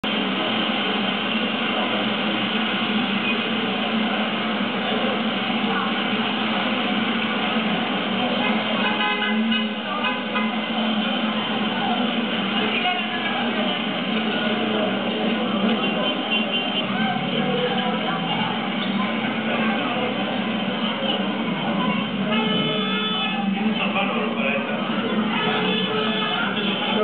Cuando el Obelisco vio la nieve
La gente enloquecida se junto improvisadamente en el centro de la ciudad como si se hubiera ganado el mundial de futbol, al lado del Obelisco rodeado de nieve, icono caracteristico de Buenos Aires